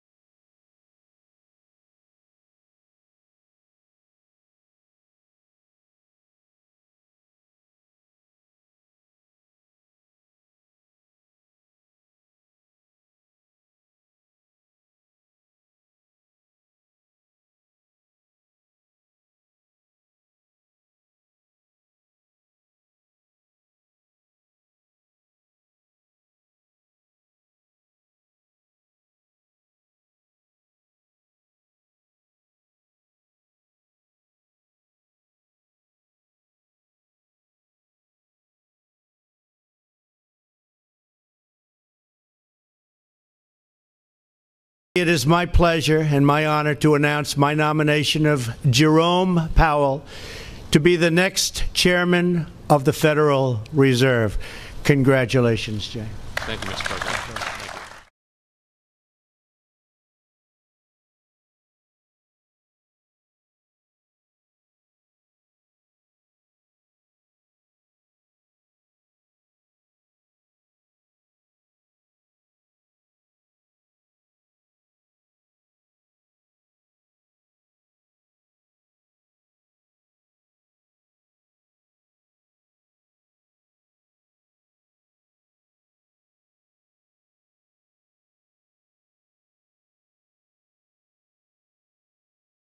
La Fed de Trump
Los cambios que vienen en el banco central estadounidense.

Imagen:
AP
C-Span

Producción:
Gabriel Arrache